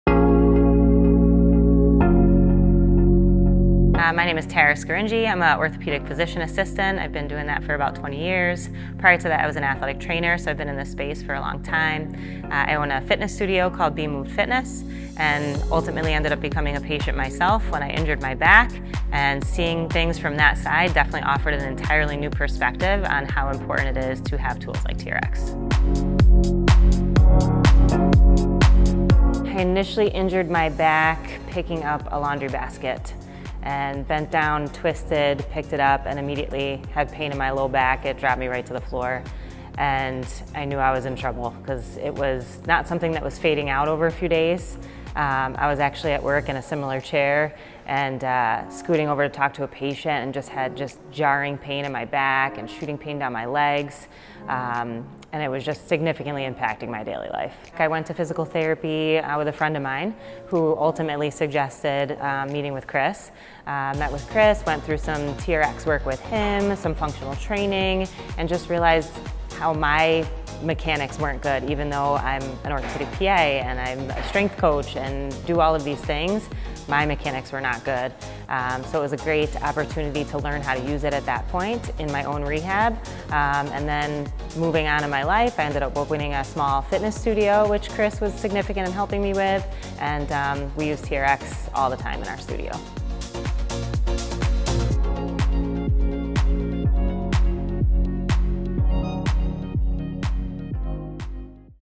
Managing Extreme Back Pain with TRX
An Orthopedic Physician Assistant and strength coach, Tara discovered a newfound respect for TRX when she injured her back.